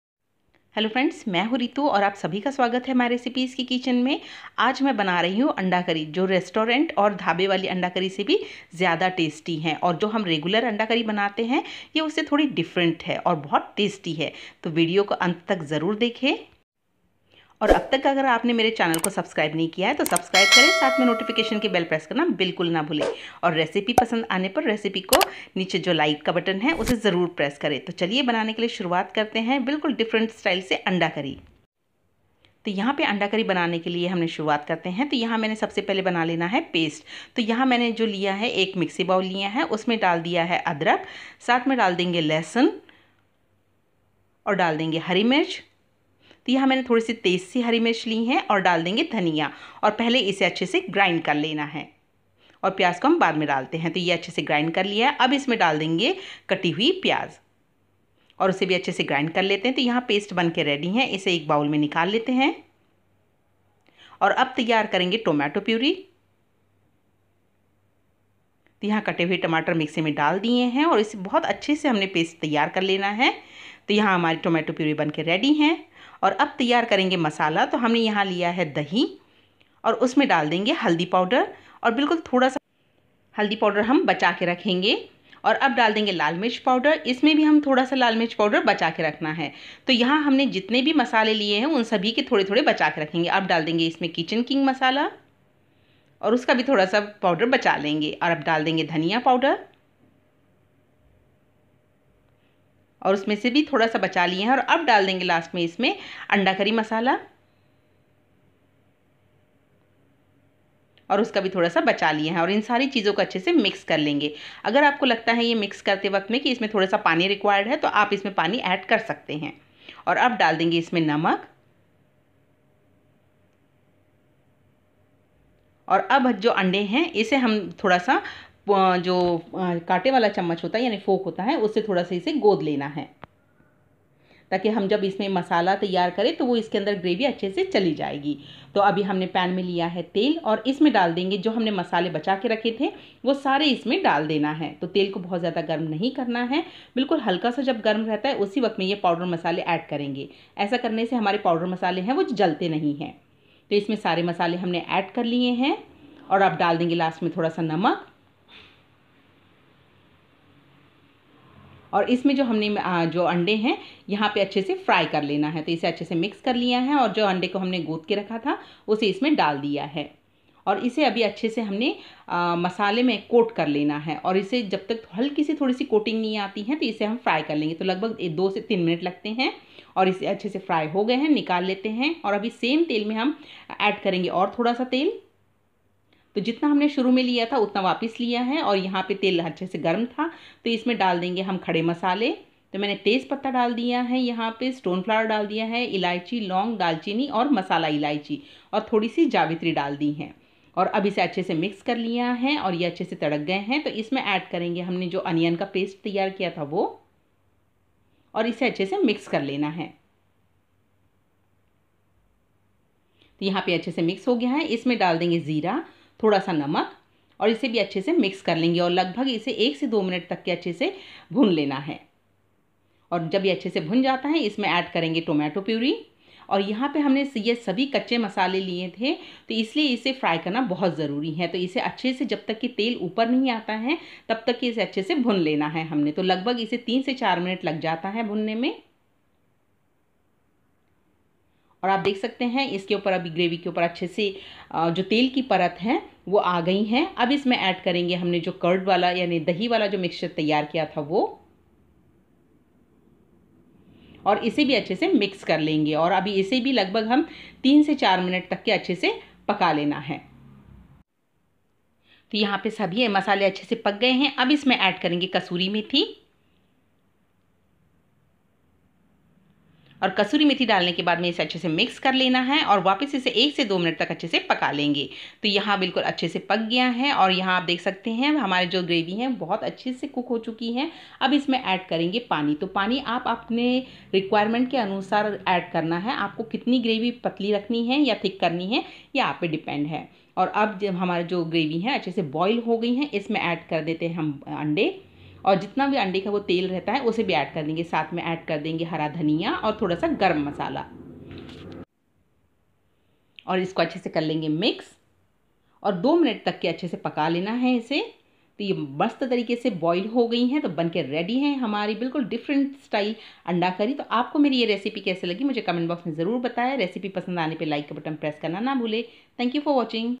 Egg Curry |रेस्टॉरेंट और धाबे से भी ज्यादा स्वादीष्ट अंडा करी बनाने का परफेक्ट तरिका | Egg Curry in.
---: Egg Curry :--
curry paste :- 👇
garlic 8/10
coriander leaves 3tbsp
onilon 2 medium
Tomato 2 medium
make Paste
Curd mixture :--👇
curd 1/4 cup
turmeric 1/4tsp
redchilli powder 1/2 tsp
kitchen king masala 1 & 1/2 tsp
egg curry masala 1tbsp
mix all ingredients and make paste
hole spice:-👇
green cardamom 2/3
black masala cardamom 1
bayleaf 1
stone flower
cinnamon 1
mace
cloves 2/3
add oil to pan add powder masala and add boiled egg fry for 2/3 min
add more oil to pan add hole spice
add onion paste cook it add cumin 1/2 tsp and salt 1/4 tsp cook wel.
add tomato paste cook wel as shown
than add curd paste again cook wel till masala relise oil than add water 1 & 1/2 cup water bring to boil add eggs , green coriander & garam masala .cook again 2min .
Keep supporting for more new videos.